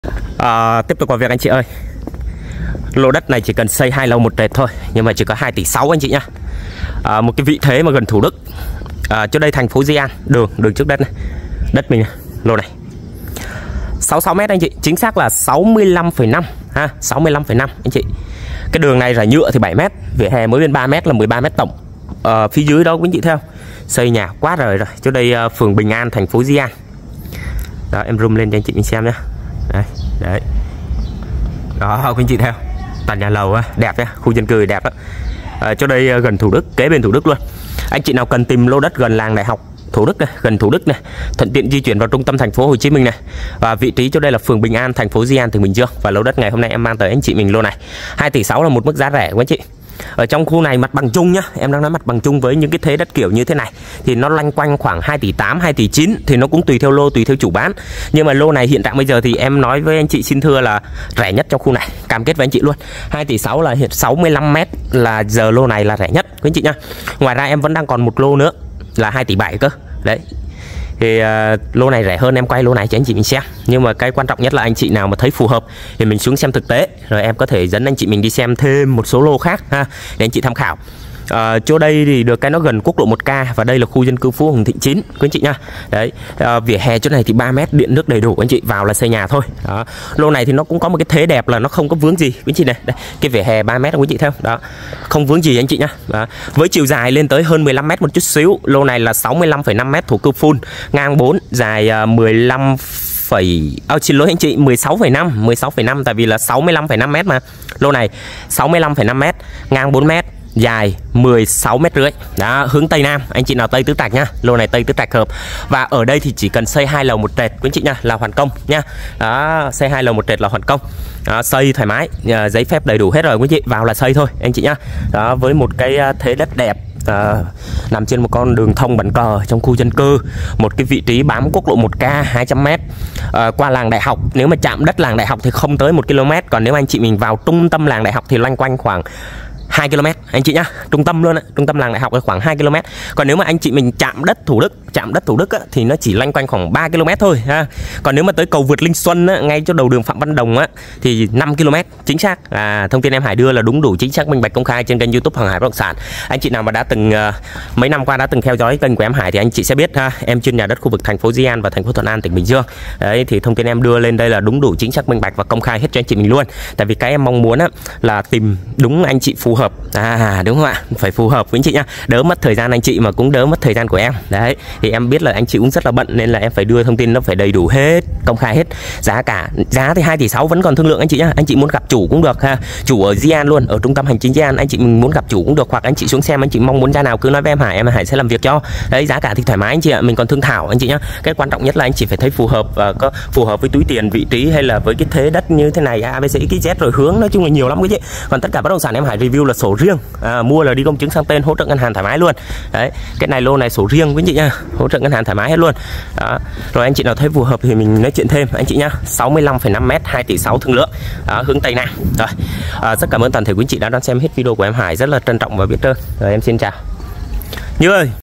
Bán Đất Dĩ An (1892) 2,6 Tỷ Gần Làng Đại Học, Khu Dân Vip , Giá Rẻ Nhất Luôn ạ
DT 65m2 thổ cư 100%, đất tại phường Bình An, Tp.Dĩ An, Bình Dương
Giá 2,6 Tỷ TL quá rẻ,